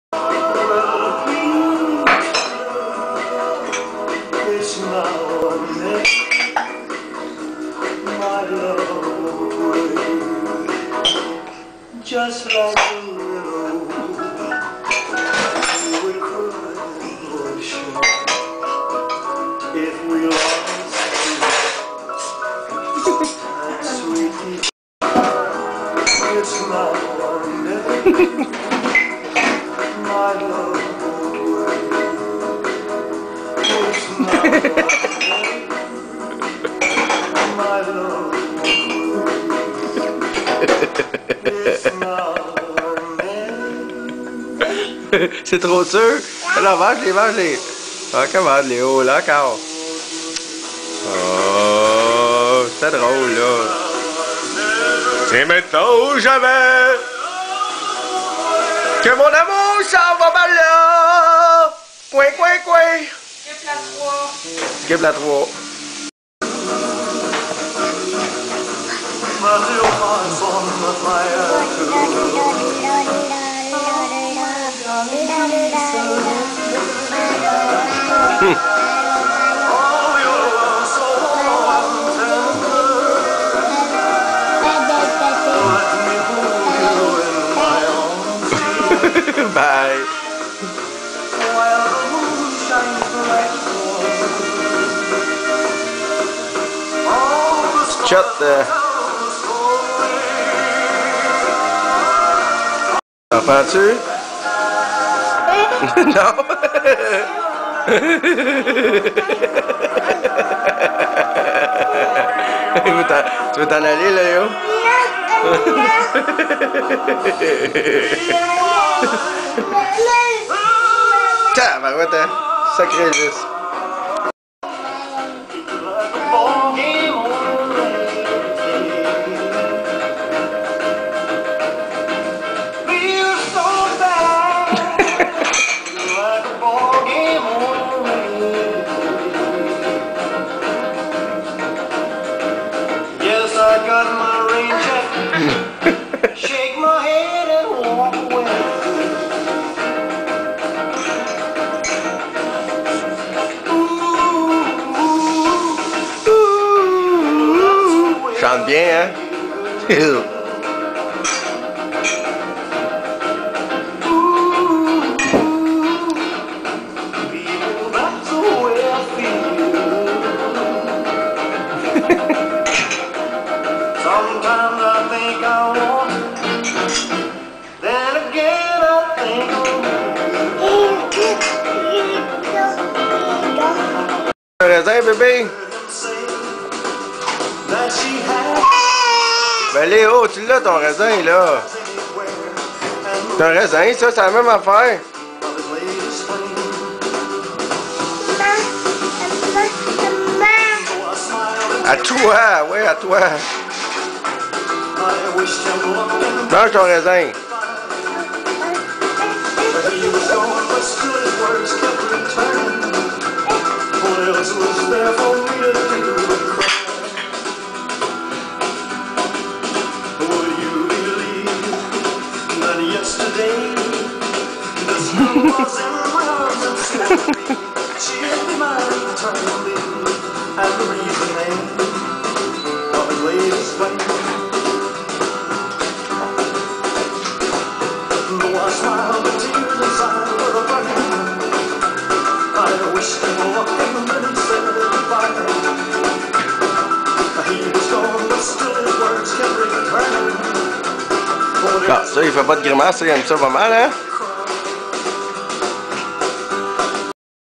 Léo mange une clémentine
Hahahahahaha! Pis y grimace en tabarouette! Ensuite, il mange une tomate cerise! Pareil! Hahahahahaha! Sur fond d'Elvis!